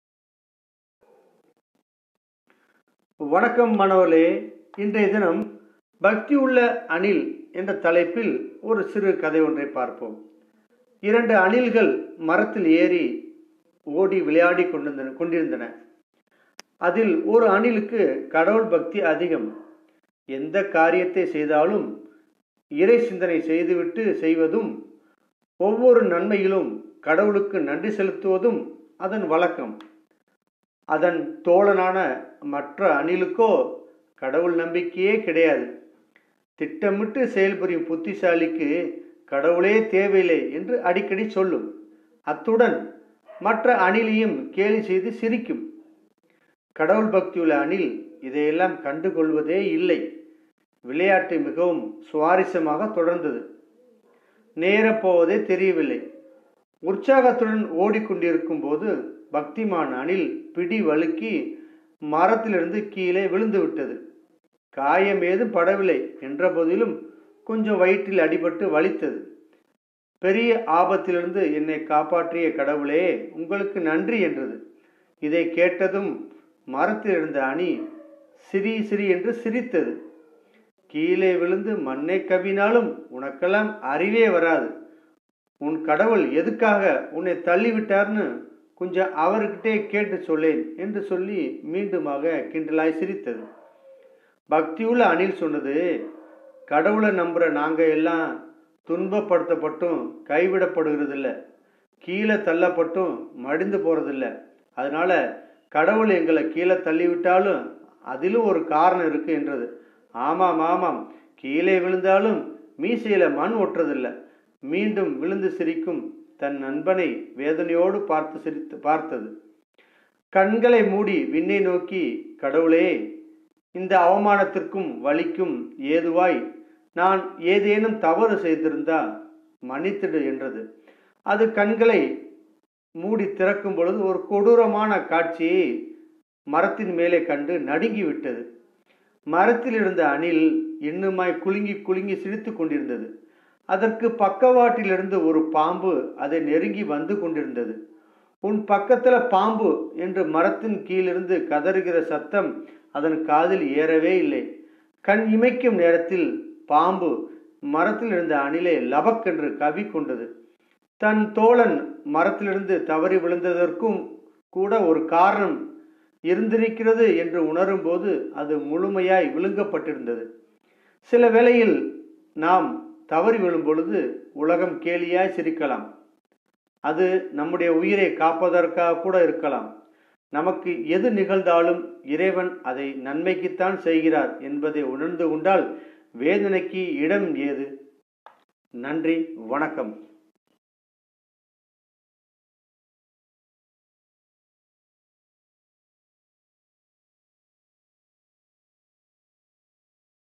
"பக்திமான் அணில்"(கதை வழிக் கற்பித்தல்)
மாணவர்களுக்கு கதைவழியாக நீதிக்கதைகளை கற்பித்தல்